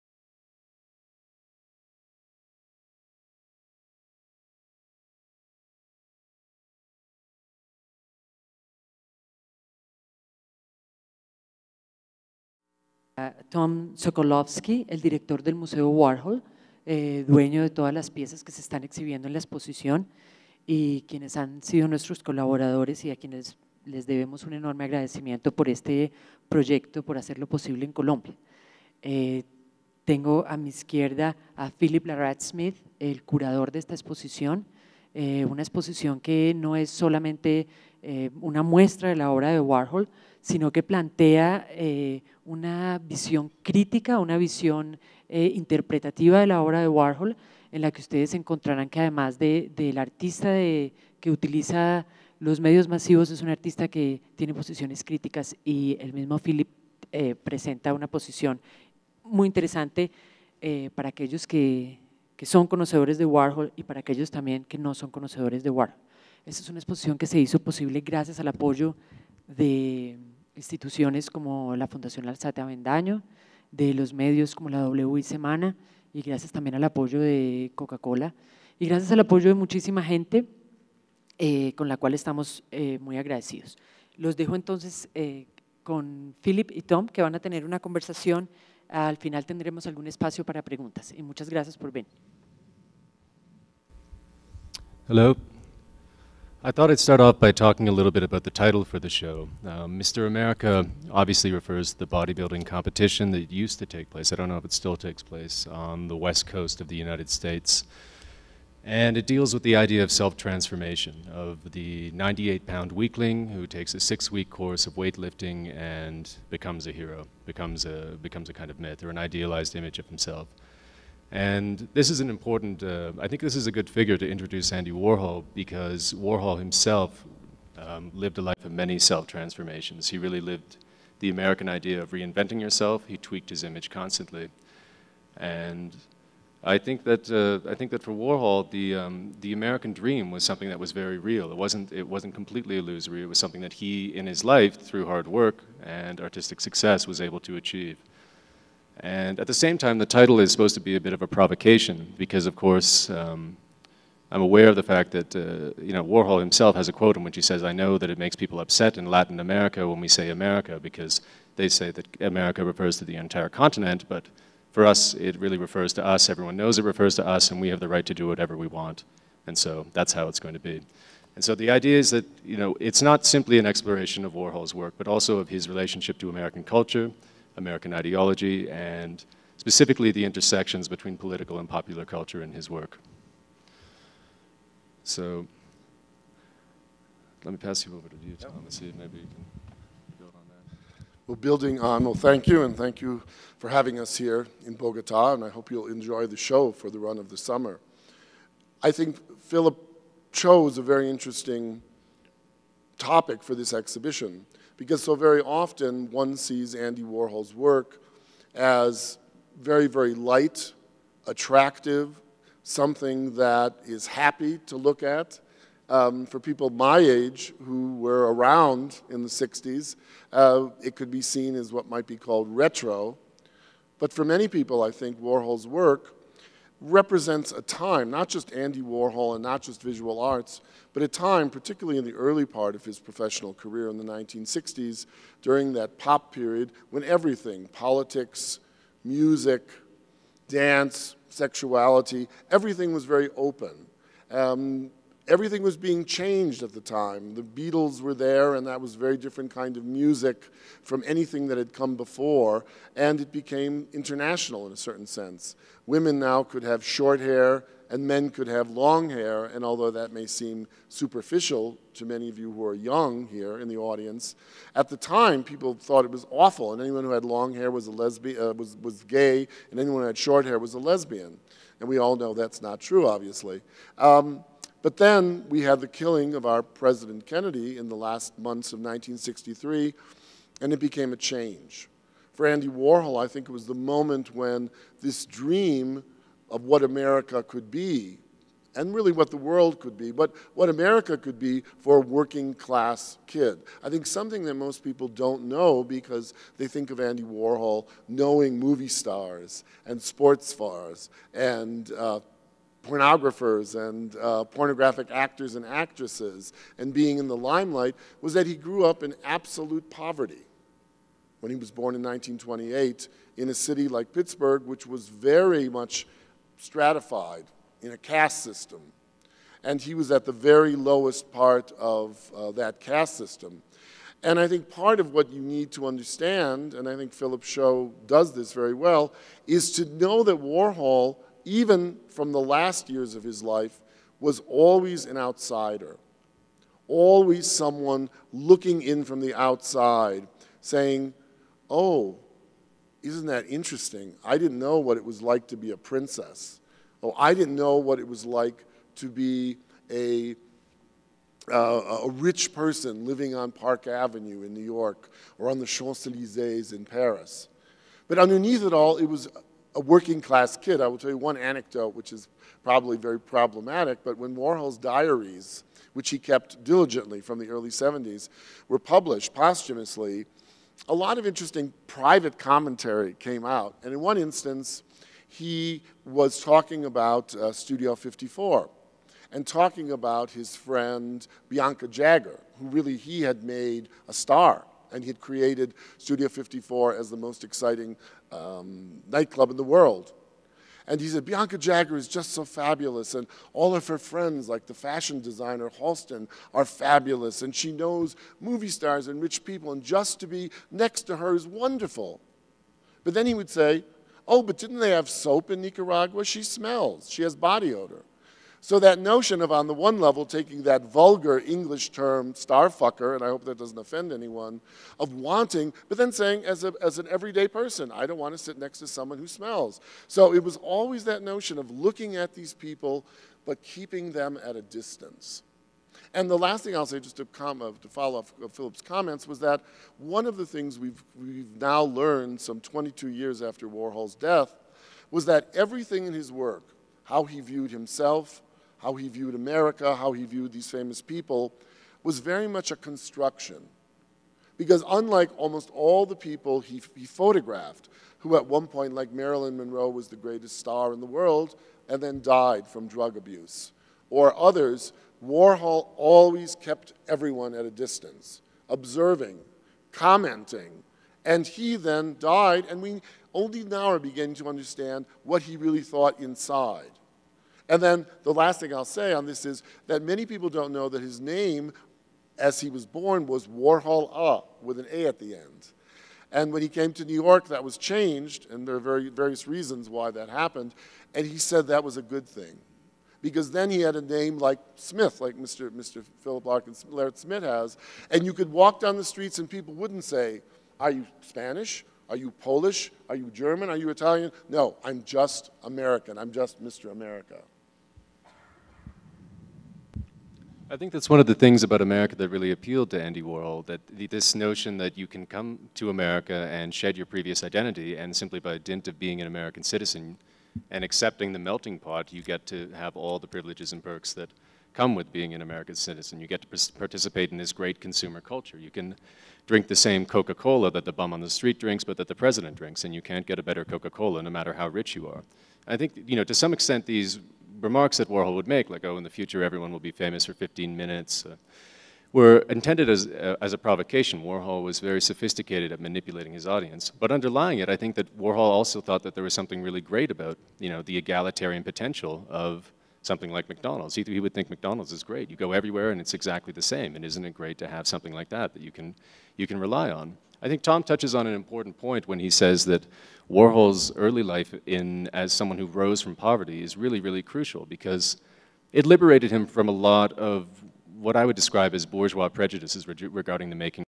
(Warhol 1/7) P. Larrat-Smith & T. Sokolowski, Bogotá.
Conferencia por:
Phillip Larrat-Smith (curador de la exposición Mr. America de Andy Warhol) & Tom Sokolowski (director del Museo Andy Warhol en Pittsburg, EE.UU) (Parte 1 de 7)